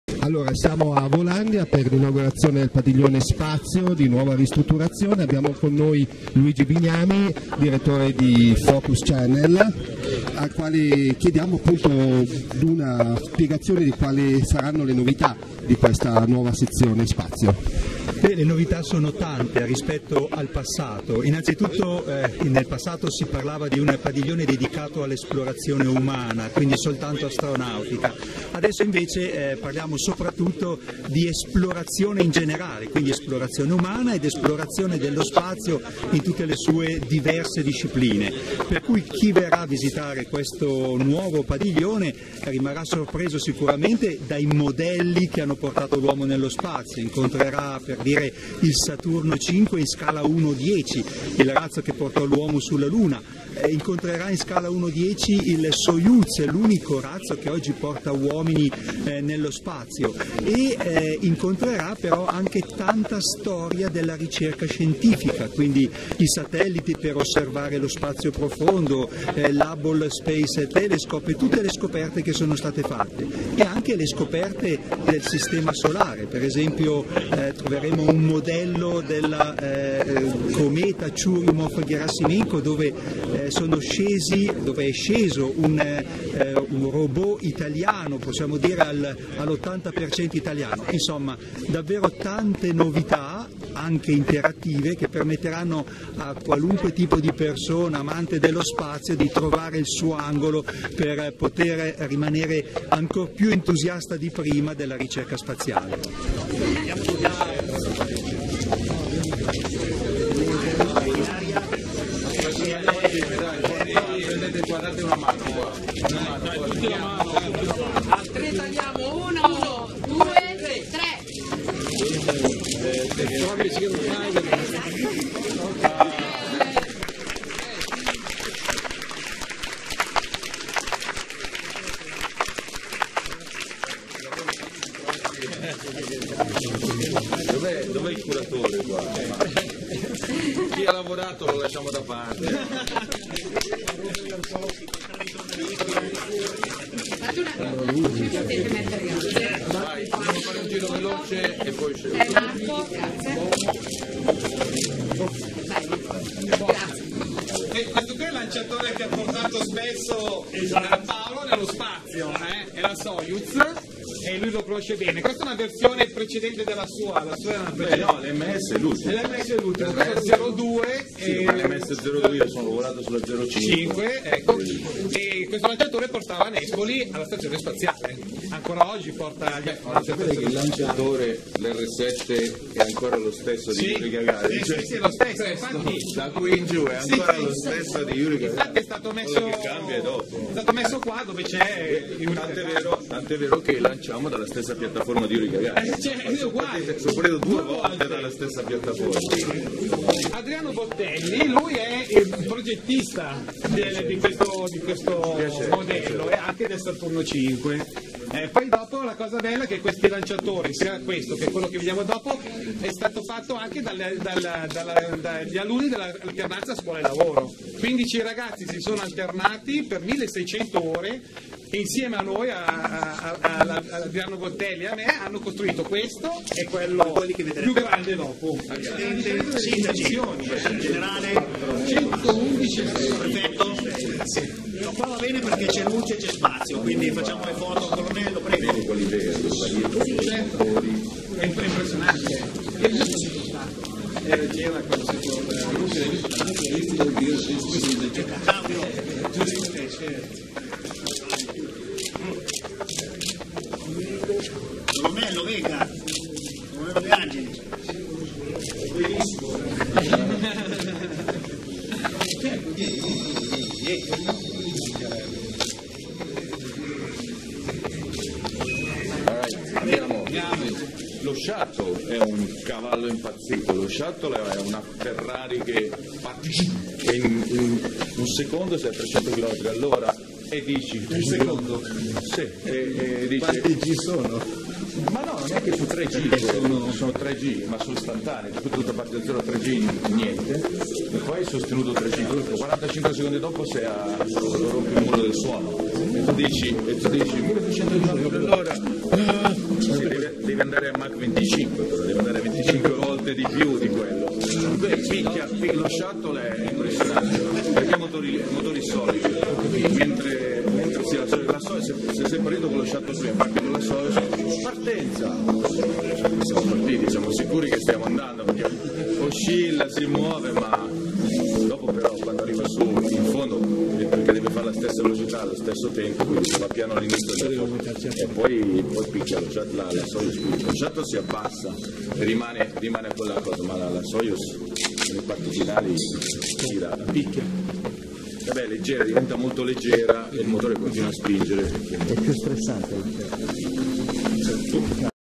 Paolo Nespoli a Volandia
Inaugurazione Padiglione Spazio Volandia 2 marzo 2019